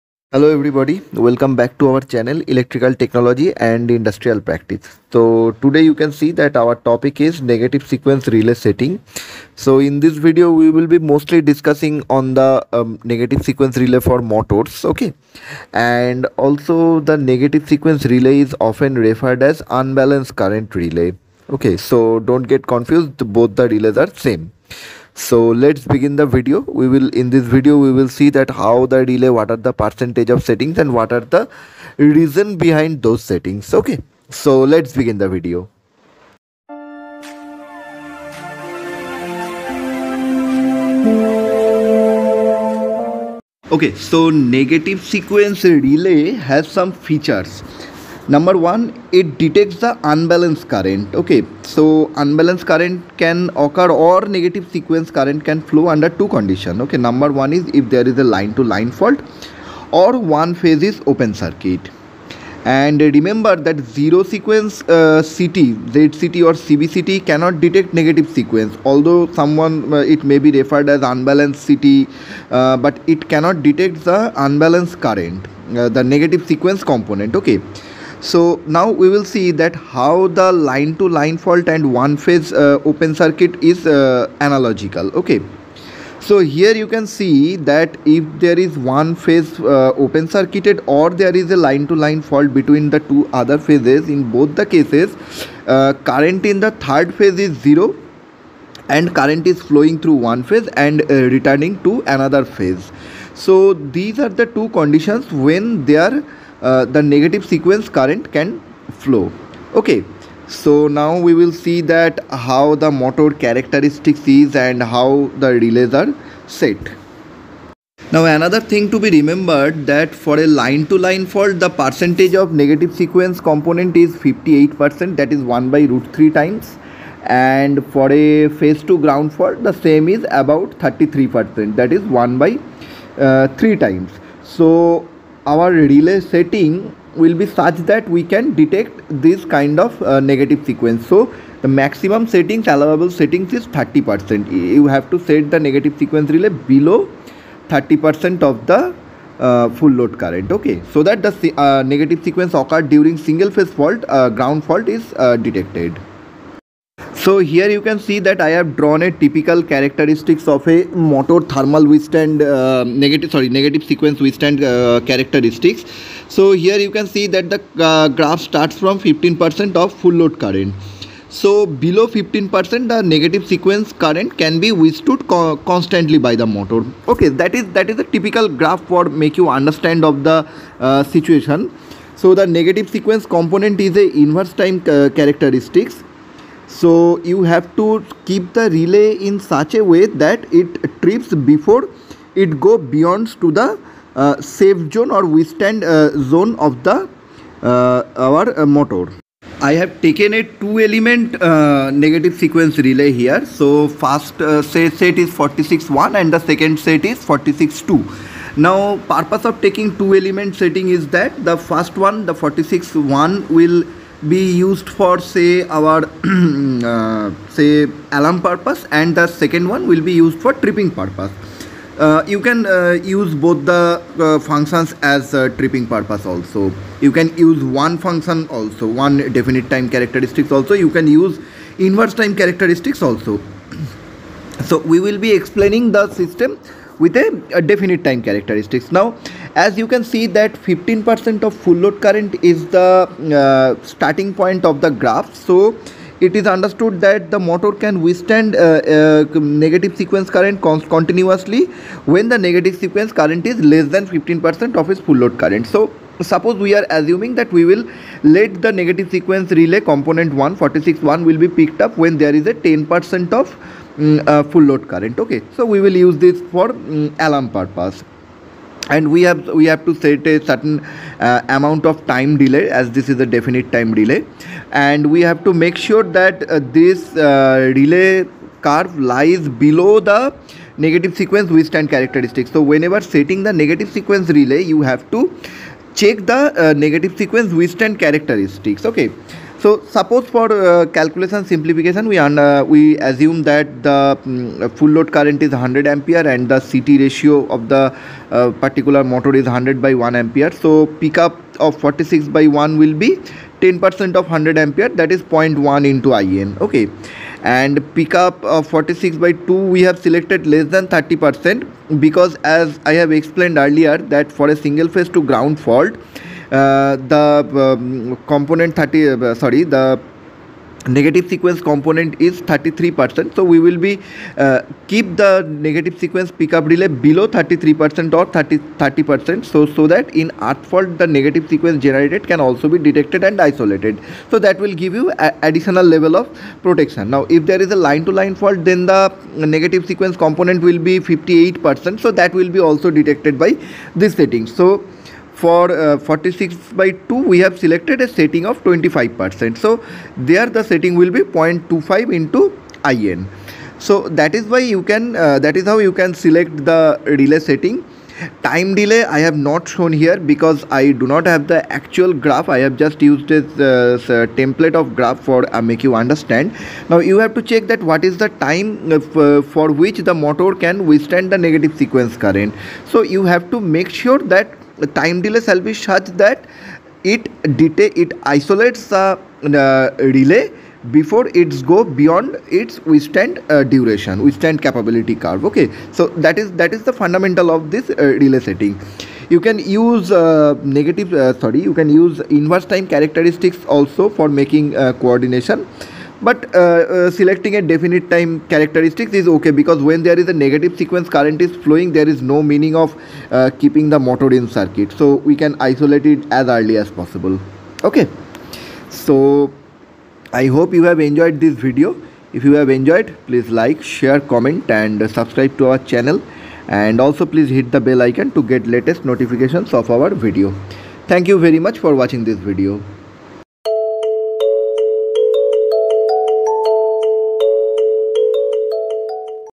Negative sequence relay setting calculation| Unbalance protection of motor
In this video we have discussed on the negative sequence relay setting of motor. Negative sequence relay is often referred as unbalance protection relay.